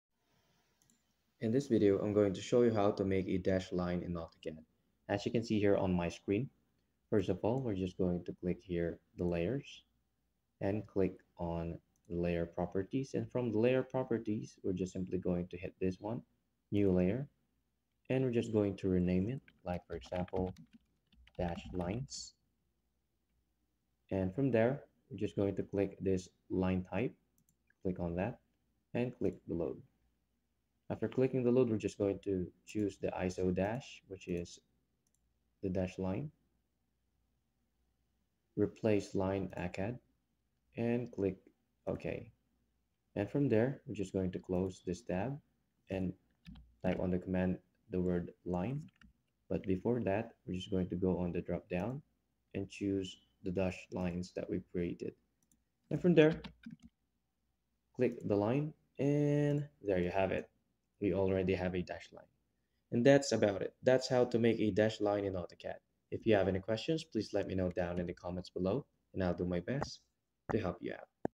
How to Make a Dashed Line in AutoCAD [QUICK GUIDE]
Let's take a look at how to make a dashed line in AutoCAD.